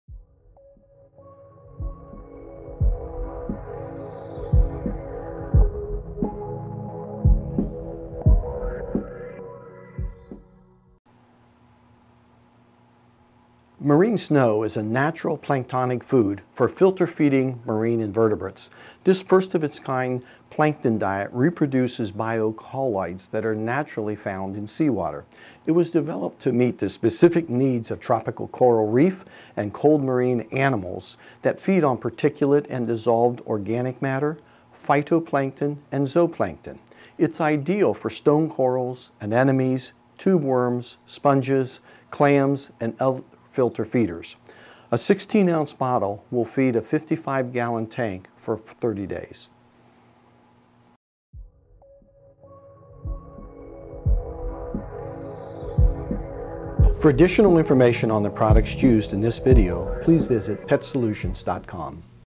PetSolutions: Marine Snow
Marine Snow is a natural planktonic food for filter feeding marine invertebrates. This first of its kind planktoon diet reproduces the special bio-colloids found in natural seawater. Developed to meet the special needs of tropical coral reef and cold marine animals that feed on particulate and dissolved organic matter, phytoplankton and zooplankton. Ideal for soft and stone corals, anemones, tube worms, sponges, tunicates, clams, and other filter feeders. 16 oz. bottle of MarineSnow will feed a 55 gallon tank for a month.